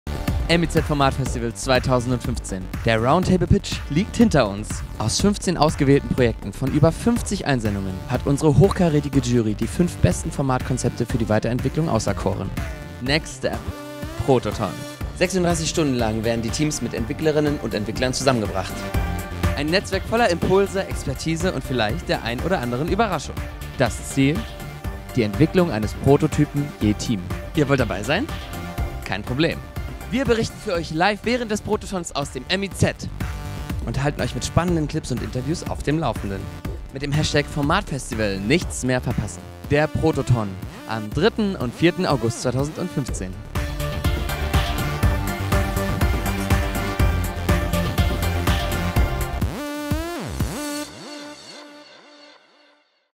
Der Protothon - dabei sein mit Formatfestival
Das MIZ-Formatfestival 2015 prämiert journalistische Formatideen zwischen TV, Radio und Onlinemedien in einem viermonatigen Entwicklungsprozess. Die Bewerbungsphase beginnt am 6. Mai und endet am 28. Juni 2015.

Pitchen.

Das MIZ-Formatfestival richtet sich an JournalistInnen, HackerInnen, Transmedia-StorytellerInnen, Format-StrategInnen, ProduzentInnen, KonzepterInnen und RundfunkrevoluzzerInnen. Überzeugen Sie die Jury und verwirklichen Sie anschließend Ihre Idee!

Aus allen Einreichungen werden am 28. Juli 2015 beim Roundtable fünf Projektideen für die Teilnahme am Formatfestival ausgewählt. Bewerben Sie sich vom 6. Mai bis zum 28. Juni 2014.

Entwickeln.

Der anschließende Protothon bietet den fünf finalen Projektteams vom 3. bis 4. August 2015 die Möglichkeit mit erfahrenen Programmieren repräsentative Prototypen aus ihren Formatkonzepten zu entwickeln.

Um den Entwicklungsprozess weiter voran zu treiben, erhalten die Projekte vom 31. August bis 1. September 2015 bedarfsorientierte Coachings und während des gesamten Entwicklungsprozesses bei der Format-Sprechstunde konstruktives Feedback.

Gewinnen.

Die finalen Prototypen und ausgearbeiteten Formatkonzepte reichen die TeilnehmerInnen bis zum 11. Oktober 2015 bei der Auswahlkommission ein.

Im Rahmen der öffentlichen Abschlußveranstaltung am 19. November 2015 prämiert die Jury im MIZ-Babelsberg das stärkste journalistische Konzept mit einem Preisgeld in Höhe von 5.000 Euro und bietet die Option auf eine MIZ-Innovationsförderung.
Die fünf besten Projekte erhalten zudem die Möglichkeit, ihre Formatpiloten, im Rahmen des von der European Broadcasting Union veranstalteten Eurovision Creative Forums vor internationalem Fachpublikum zu präsentieren.

Die Jury.

Die Auswahlkommission des MIZ-Formatfestivals setzt sich zusammen aus erfahrenen SendervertreterInnen, ProduzentInnen, FormatentwicklerInnen und VertreterInnen von Produktionsfirmen mit internationalem Wirkungskreis.
Die JurorInnen des MIZ-Formatfestivals sind:

Christian Beetz
Geschäftsführer, gebrueder beetz filmproduktion

Dr. Bettina Brinkmann
Head of TV, European Broadcasting Union

Ernst Feiler
Head of Technology, UFA SERIAL DRAMA

Friedrich Küppersbusch
Geschäftsführer, probono fernsehproduktion

Kay Meseberg
Arte

Dr. Ralf Müller-Schmid
Leiter, DRadio Wissen

Petra Schmitz
Programmkoordination ZAP & Programmentwicklung FS, rbb Fernsehen

Simon Sturm
Programmbereich Internet, Westdeutscher Rundfunk

Panagiotis Trakaliaridis
strategische Programmentwicklung, ZDF

Julius Tröger
Leiter des Interaktiv-Teams, Berliner Morgenpost

Thomas Wallner
Gründer und Inhaber, DEEP Inc.